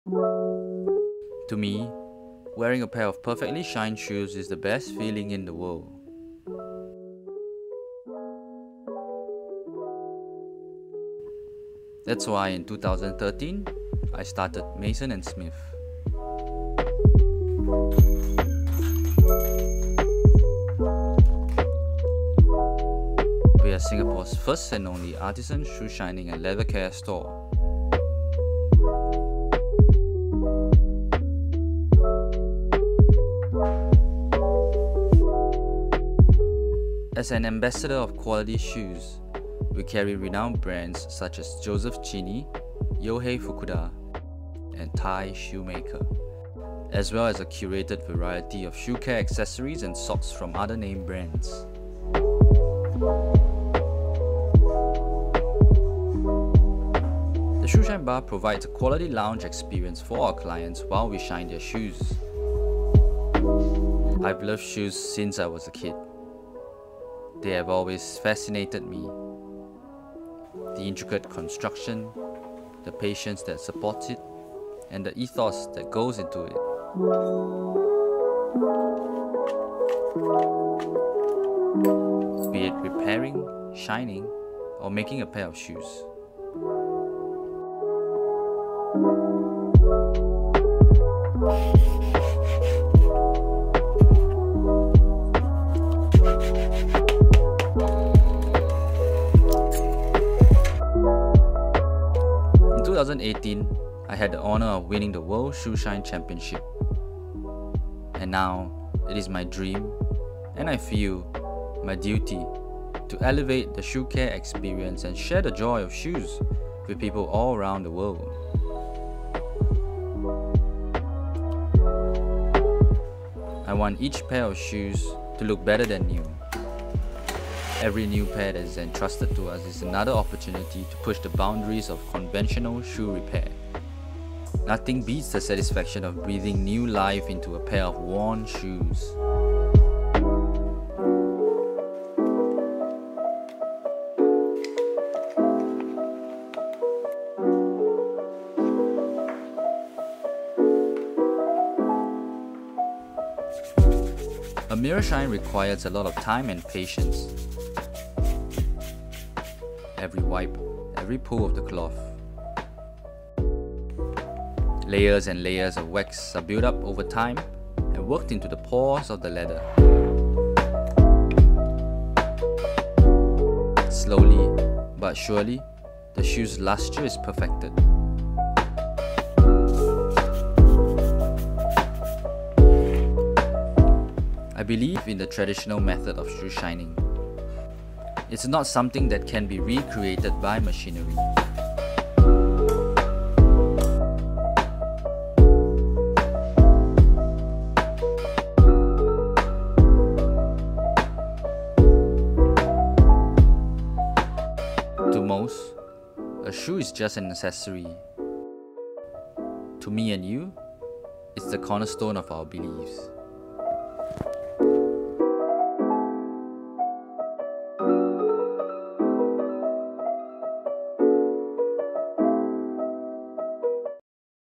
Mason and Smith - More Than Just A Shoe
Mason & Smith is Singapore’s first artisan shoe shining and leather care store that grew from an obsession with restoring vintage men’s shoes. Taking pains to preserve the traditional methods of shoe polishing, we are able to provide excellent and professional leather restoration services. Each and every piece of leather is polished strictly by hand and treated with the best leather care products available to lovingly restore its deserved lustre.

Make sure to select 1080p under video quality settings!

Special thanks to the following people who have helped direct and edit the above video : Amanda , Joshua “shenn” tan, Wang Kang Rui, Ash, Jay

Credits to Kirby Allison for the World Shoe Shine Championship Footage